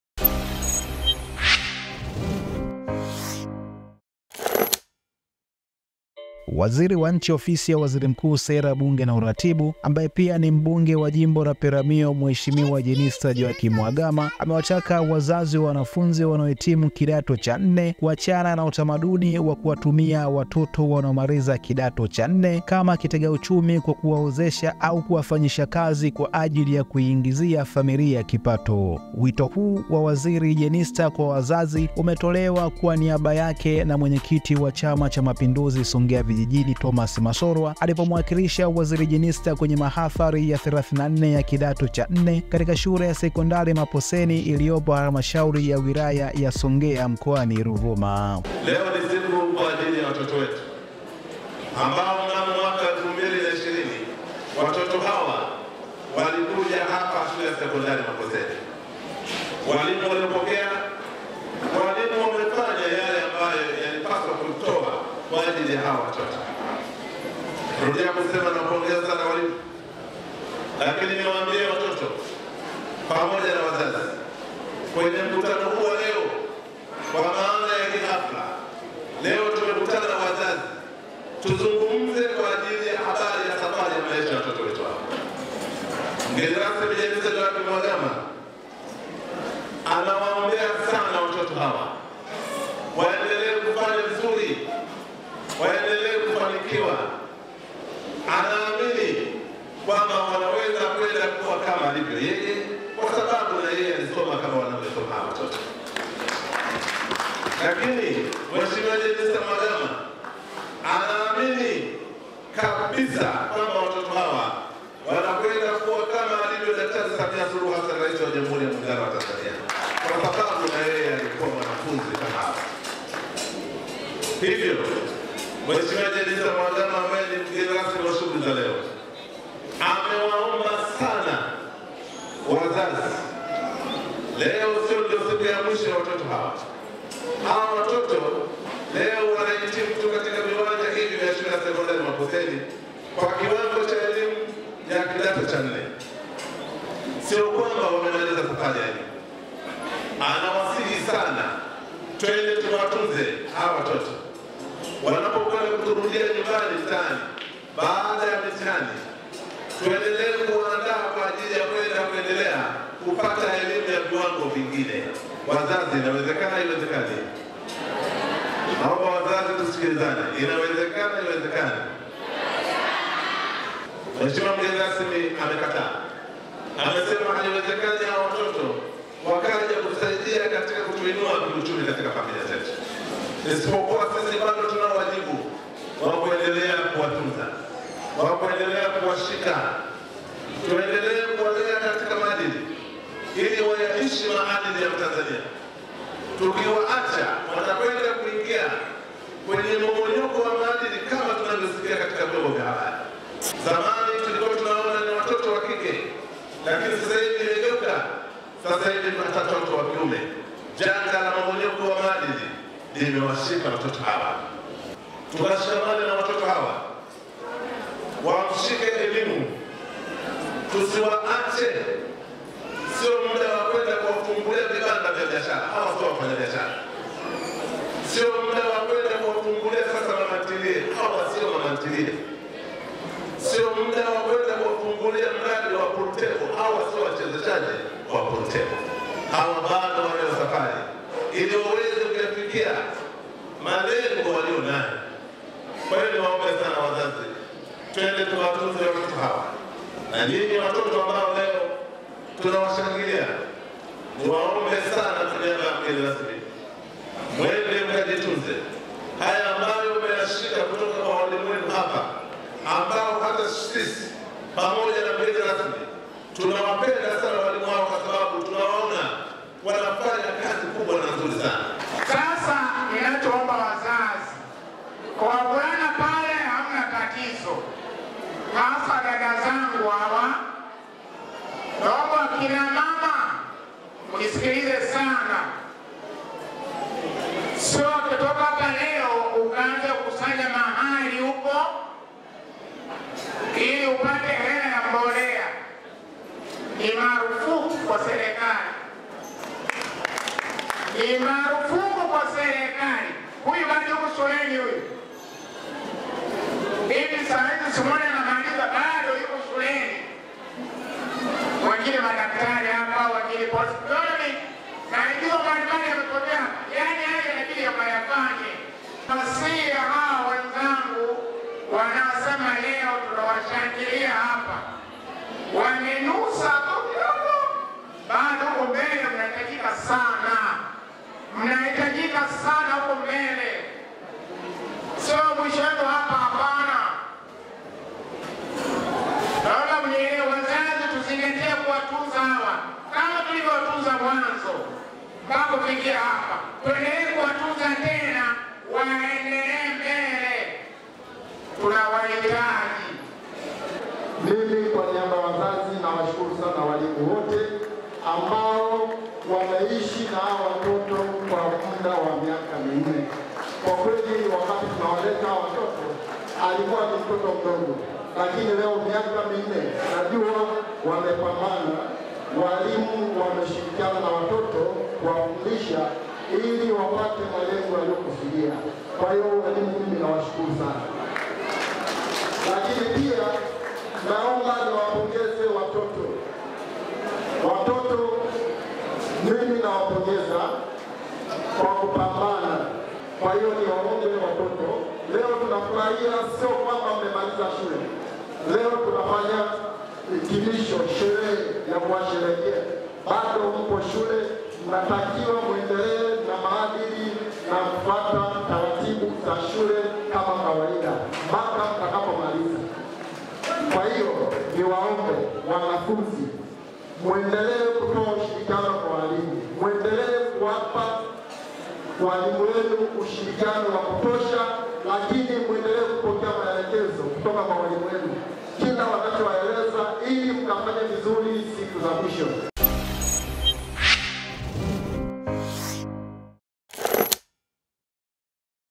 🔴 MAHAFALI YA 34 MAPOSENI SEKONDARI WAZAZI WAASWA KUTOWATUMIA WATOTO KAMA KITEGA UCHUMI
Waziri Wa Nchi Ofisi Ya Waziri Mkuu Sera, Bunge Na Uratibu Ambaye Pia Ni Mbunge Wa Jimbo La Peramiho Mhe Jenista Mhagama Amewataka Wazazi Wa Wanafunzi Wanaohitimi Kidato Cha Nne Kuachana Na Utamaduni Wa Kuwatumia Watoto Wanaomaliza Kidato Cha Nne Kama Kitegauchumi Kwa Kuwaozesha Au Kuwafanyisha Kazi Kwaajili Ya Kuiingizai Familia Kipato

Wito Huu Wa Waziri Jenista Kwa Wazazi Umetolewa Na  Wenyekiti Wa Chama Cha Mapinduzi Songea Vijijini Thomas Masolwa Alipomwakilisha Waziri Jenista Kwenye Mahafali Ya 34 Ya Kitado Cha Nne Katika Shule Ya Sekondari Maposeni Iliyopo Halmashauri Ya Wilaya Ya  Songea Mkoani Ruvuma